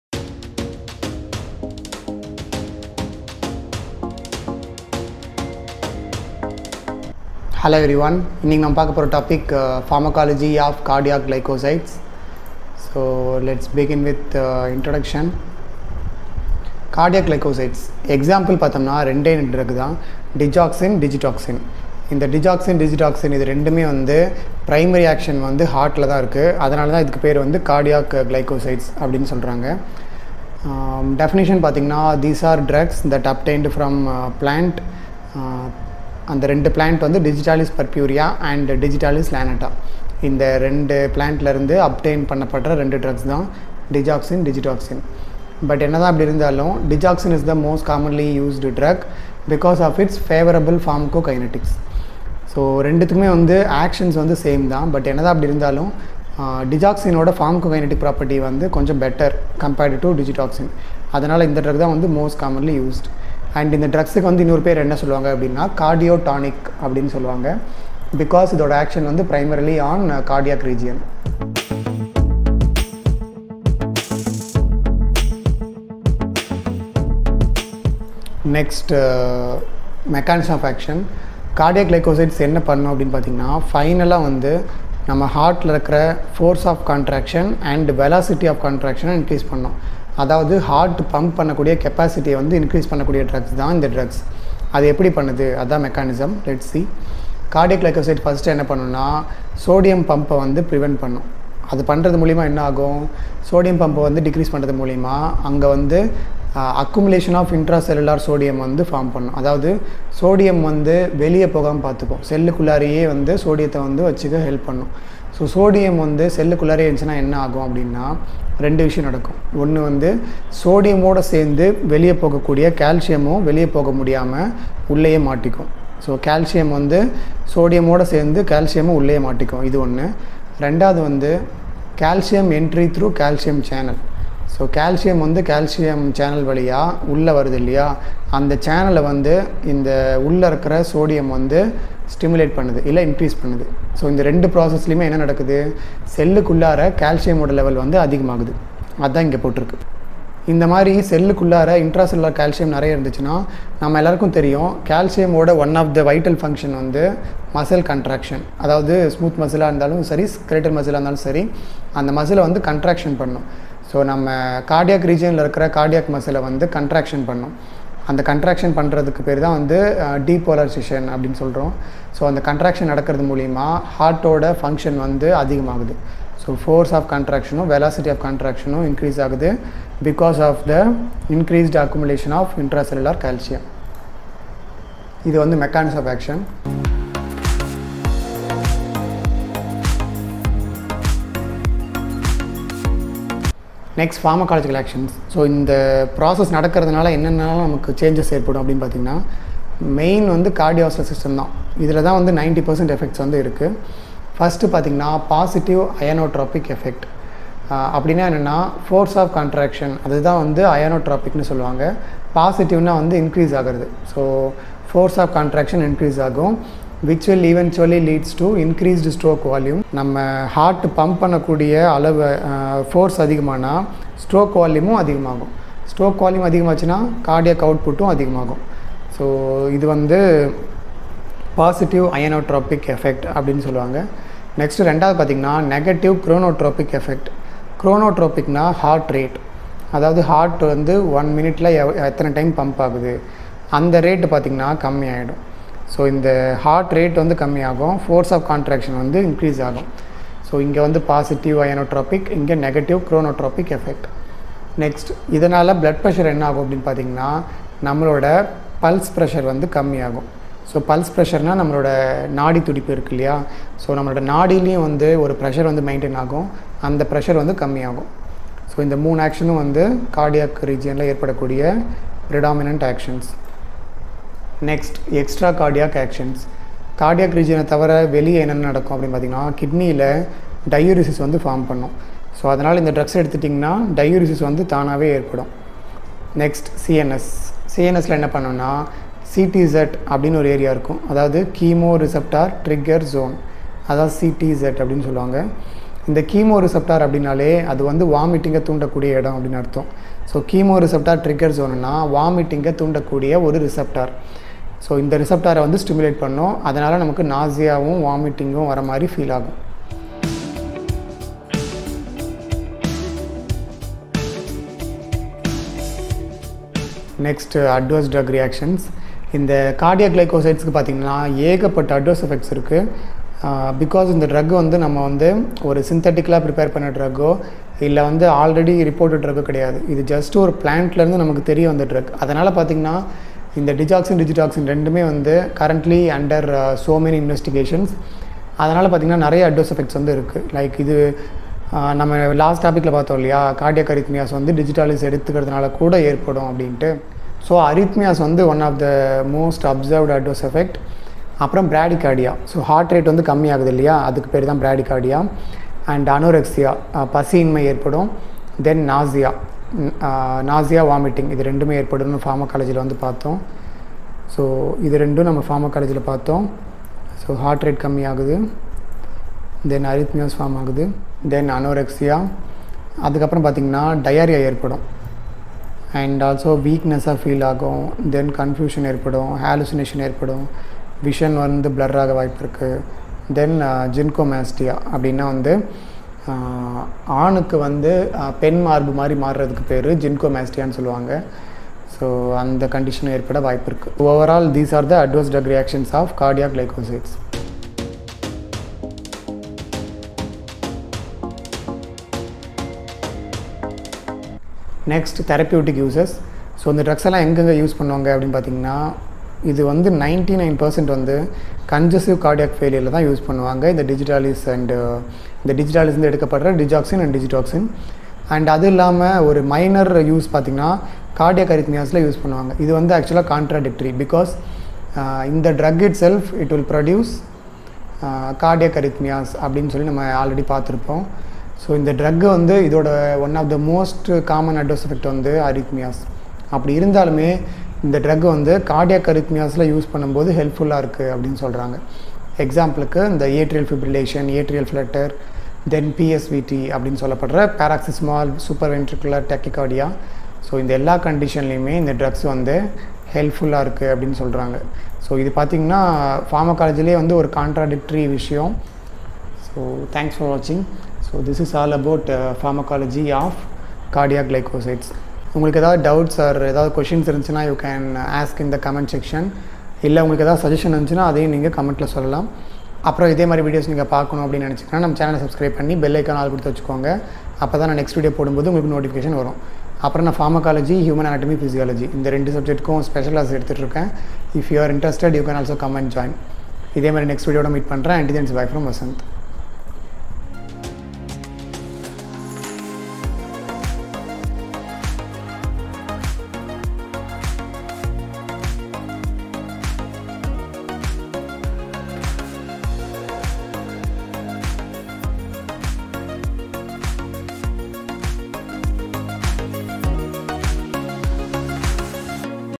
Cardiac Glycosides Pharmacology in Tamil
Highlights
Basic - 0:00
Introduction - 00:08
Classification - 02:44
Pharmacological Actions - 03:42
ADR - 04:20
Therapeutic Uses - 05:25